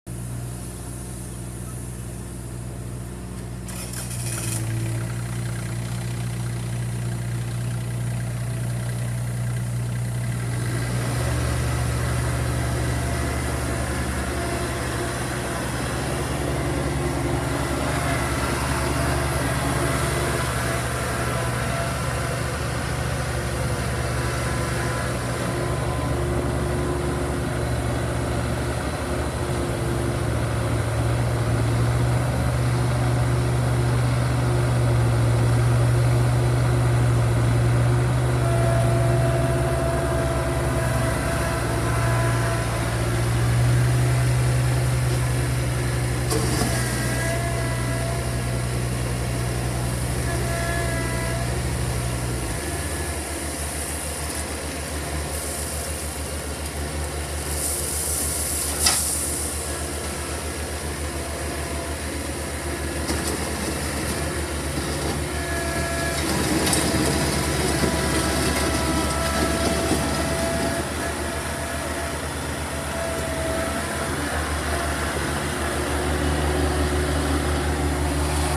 2023 BOBCAT CT1025 For Sale
OPEN CAB, INDUSTRIAL TIRES, HYDROSTATIC TRANSMISSION, 4WD, 24 HP DAEDONG DIESEL ENGINE, 46" SMOOTH BUCKET, FRONT LOADER W/ COUPLER, 540 PTO (SHOWING 2 HRS.), SN B54914589 ST# 8869